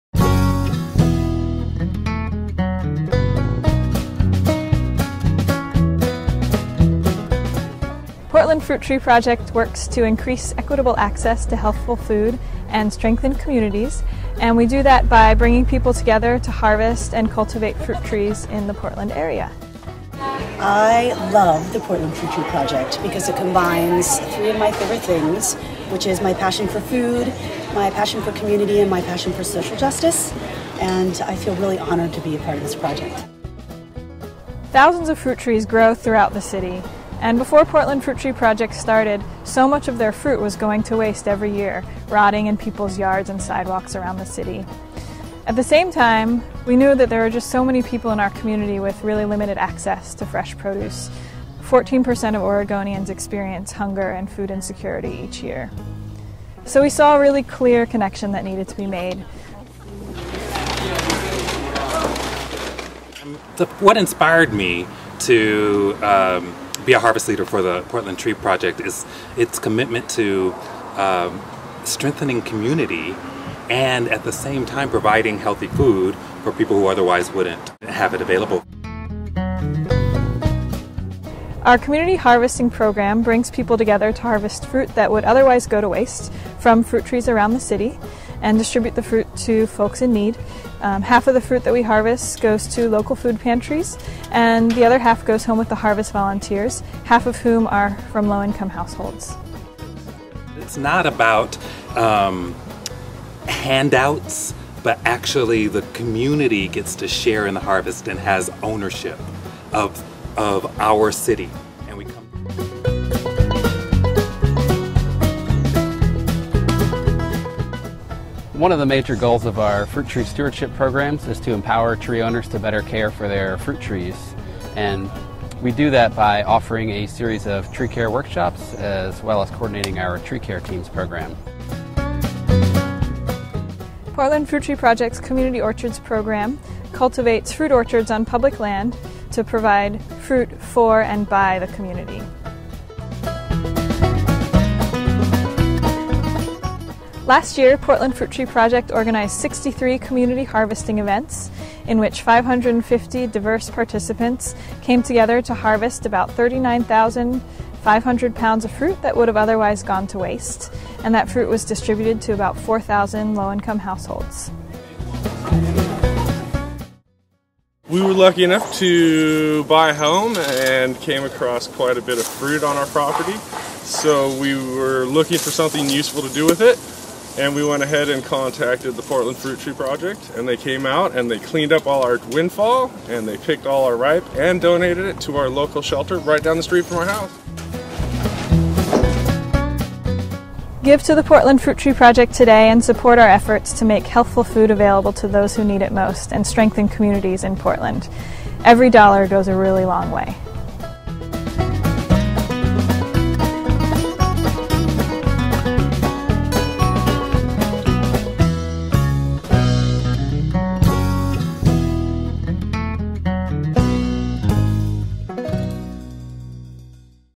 Portland Fruit Tree Project Give!Guide 2012
Portland Fruit Tree Project's mission is to increase equitable access to healthy food and strengthen communities by empowering neighbors to share in the harvest and care of city-grown produce.